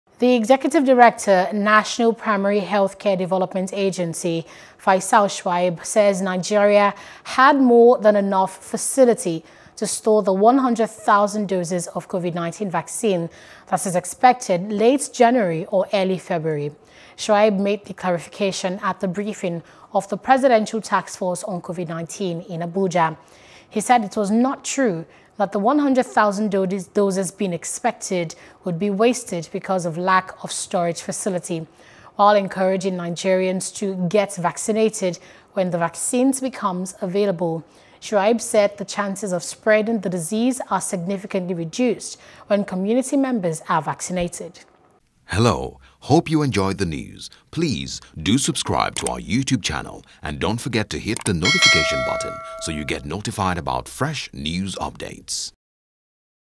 Nigeria Has Enough Facilities to Store 100,000 Doses of COVID-19 Vaccine –NPHCDA | NEWS
The Executive Director, National Primary Health Care Development Agency, Faisal Shuaib, says Nigeria had more than enough facility to store the 100,000 doses of COVID-19 vaccine being expected late January or early February.